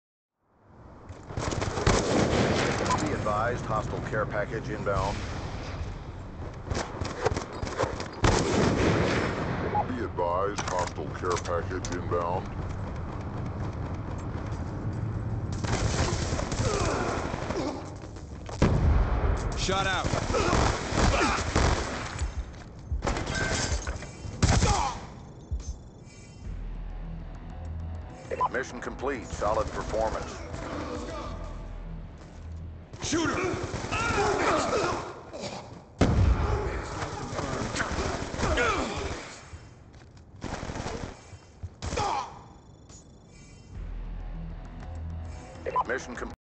4 piece killcam
Nice quickscope followed by a 4 piece final killcam